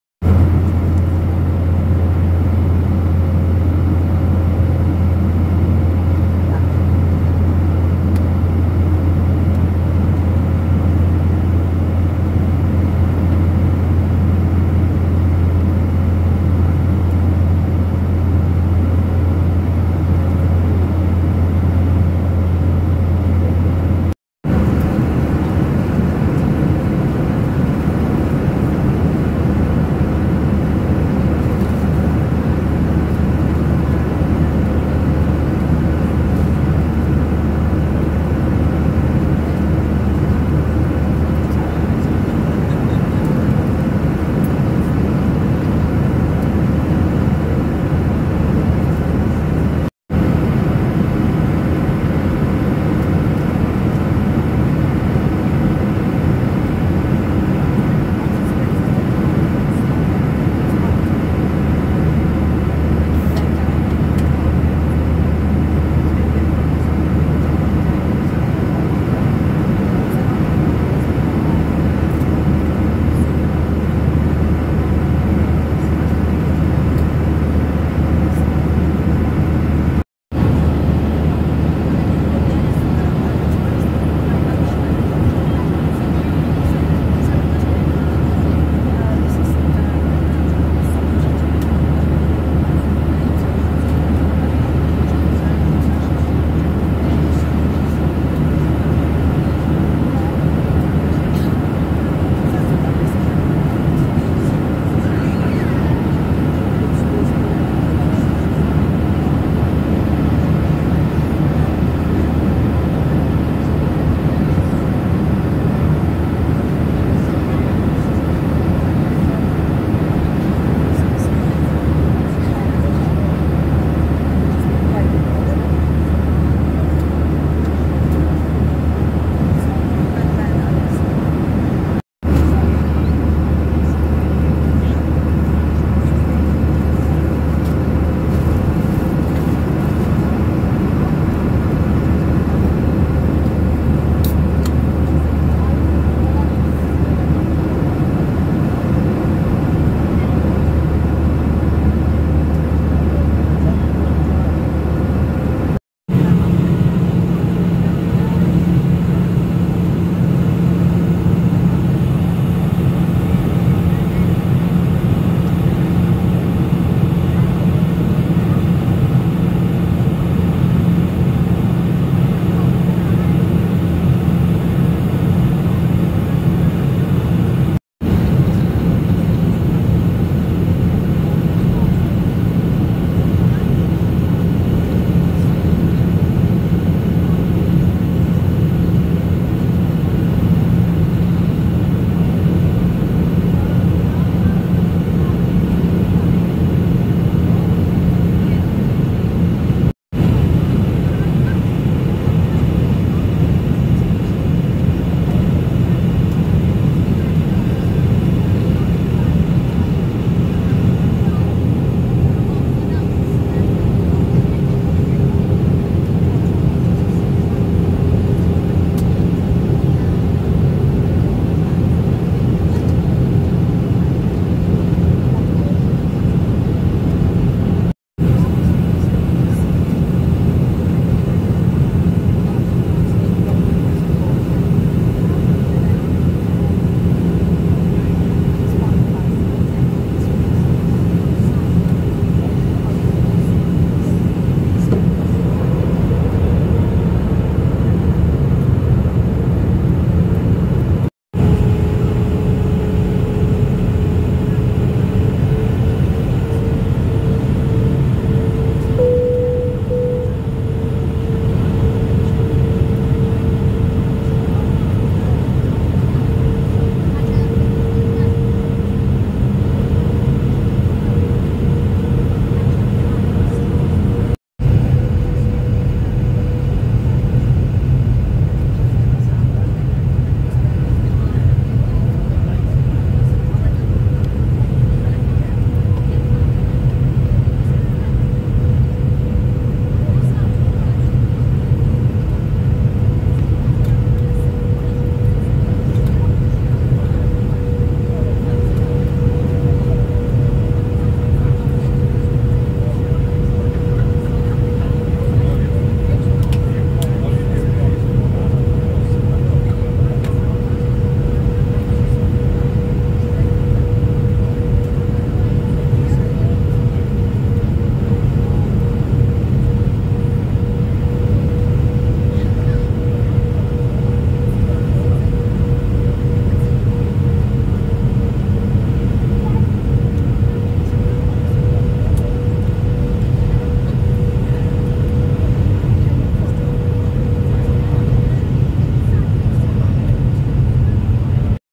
Newcastle To Zakynthos On Jet2.
Newcastle to Zakynthos on Jet2 on Saturday 25 May 2024.